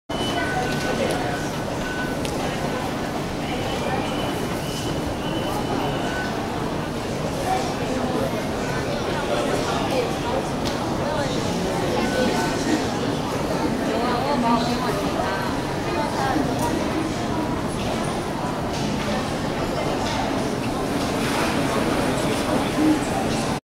People waiting for Metro 16
Waiting for metro, clock, minutes due to arrival. Unedited.